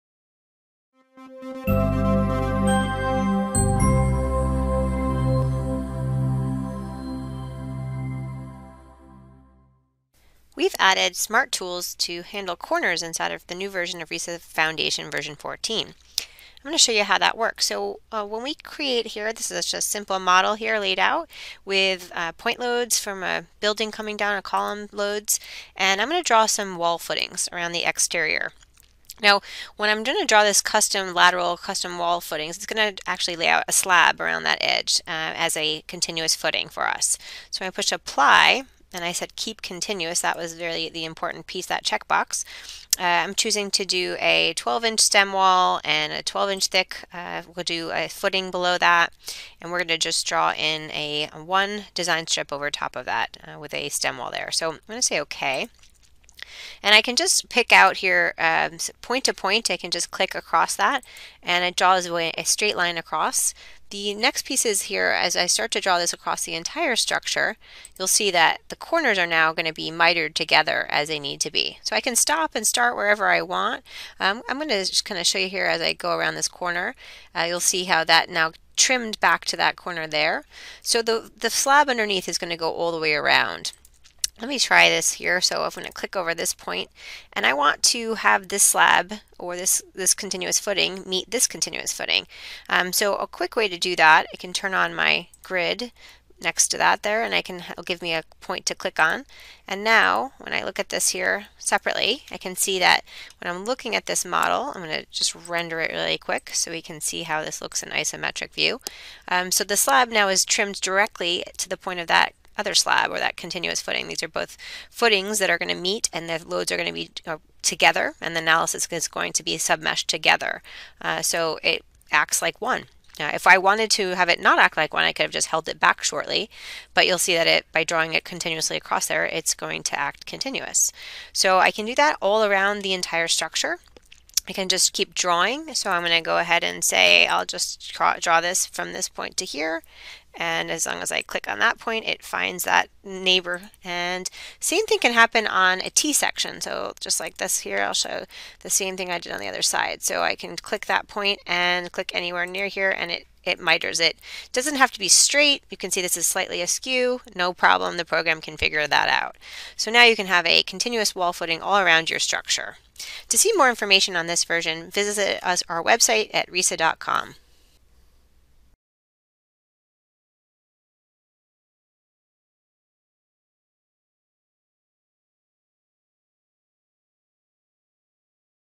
Improved Modeling of Wall Footing Corners in RISAFoundation
The new Smart Corners feature in RISAFoundation Version 14 allows for the  ability to draw continuous footings without causing overlapping slabs at the corner. To implement this, check the "Keep Wall Continuous" option when drawing a Lateral/Custom Strip Footing.